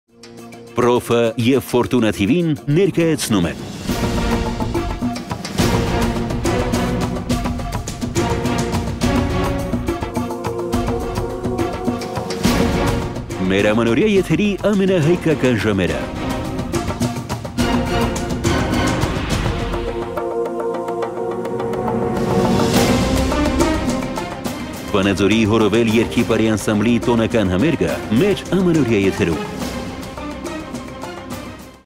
«ՀՈՐՈՎԵԼ» Ֆորտունա հեռուստաընկերության ամանորյա եթերում
Պռոֆը  և ֆորտունան  ներկայացնում են:
Վանաձորի  ,,Հորովել,, երգի պարի անսամբլի տոնական համերգը դիտեք  Ֆորտունա հեռուստաընկերության ամանորյա եթերում..